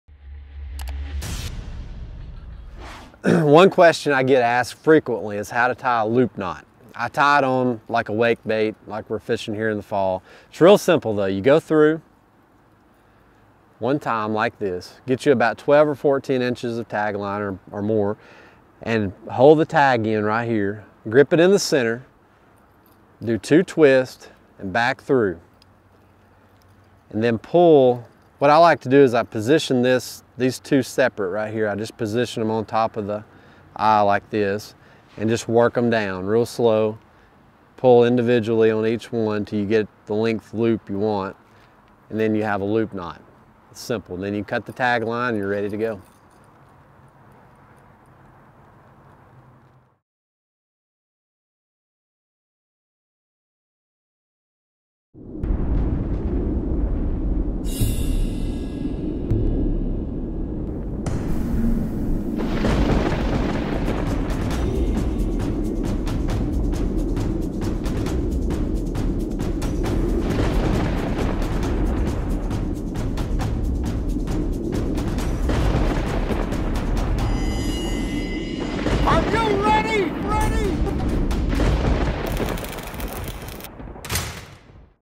How to Tie a Simple Loop Knot
Jonathan Newton walks through step by step how he ties his favorite loop knot for bass fishing topwater lures.